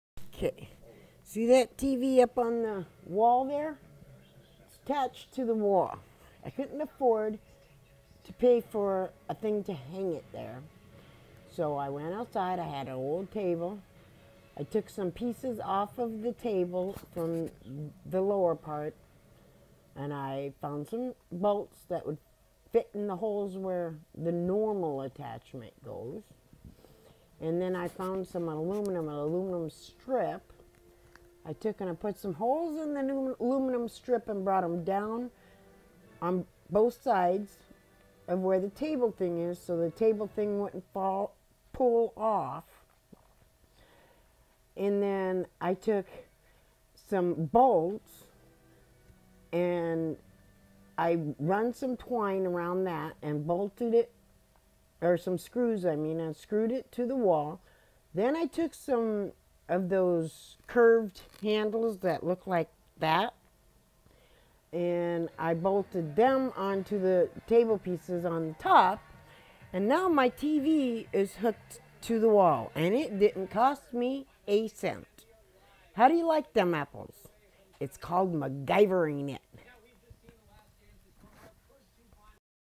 McGievered my tv to the wall
attached my tv to my wall wit some bolt screws pieces from a old table and som aluminum strips from a old window shade. not bad hu